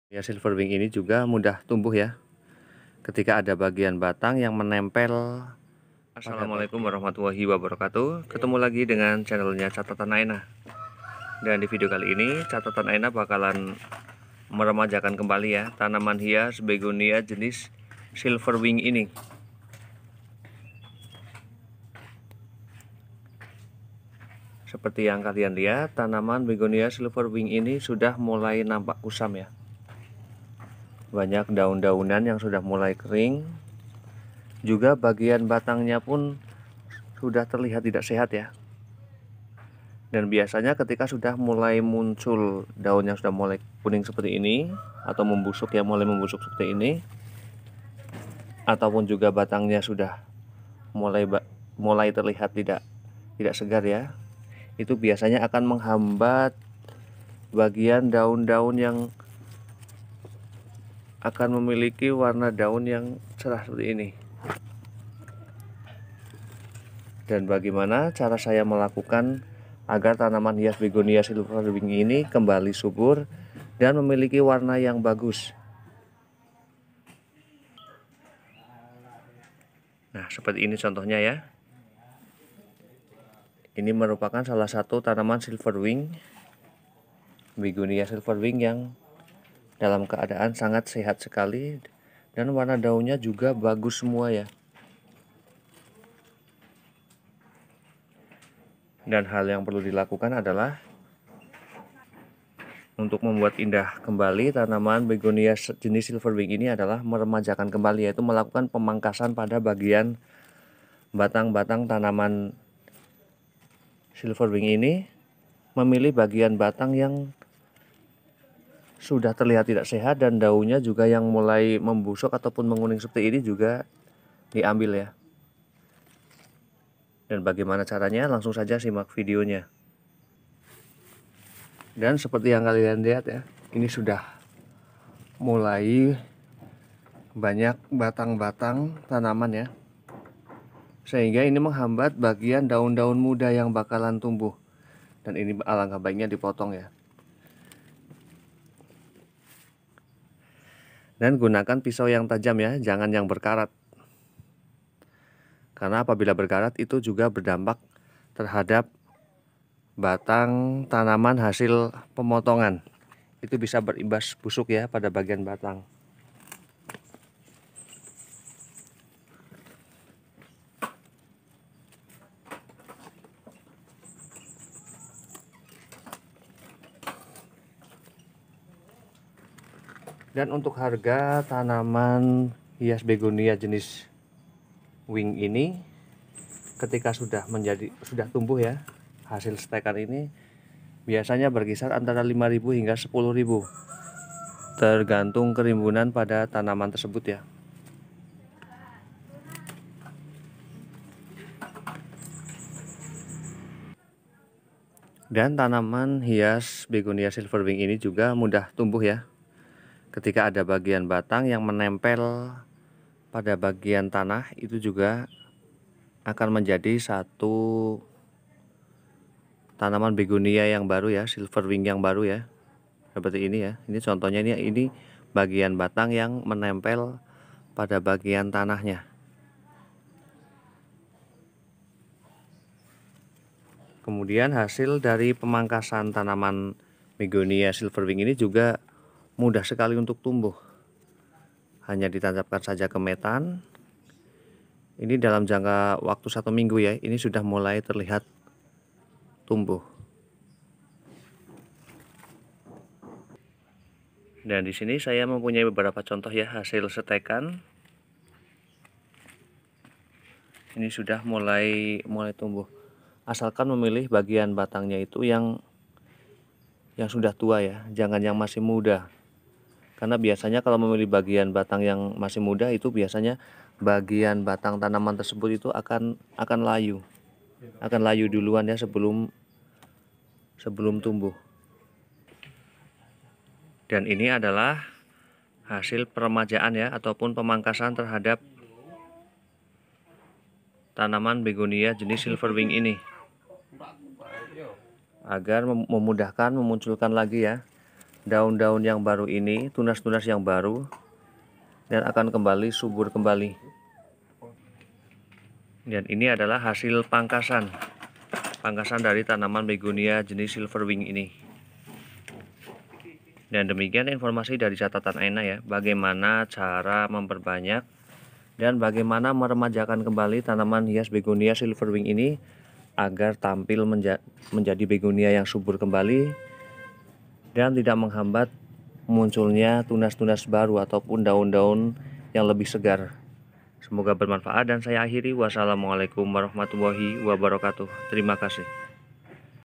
MEREMAJAKAN TANAMAN BEGONIA SILVER WINGS|PERAWATAN TANAMAN BEGONIA|BIKIN BEGONIA CANTIK KEMBALI
Setiap tanaman hias akan selalu memiliki daya tariknya sendiri bagi para pencinta tanaman hias. Bahkan, bisa dibilang kalau hampir setiap orang akan selalu tertarik atau terpikat dengan tanaman hias. Banyaknya orang yang tertarik untuk melihat tanaman hias atau merawat tanaman hias itu sendiri karena tanaman hias sangat identik dengan keindahan, mulai dari daunnya, batangnya, hingga bunga dari tanaman hias.
dan divideo kali ini catatan Aina bakal kasih tips agar tanaman hias begonia makin tampil cantik dan indah.
Langsung saja kita simak videonya.
semoga bermanfaat
dan terimakasih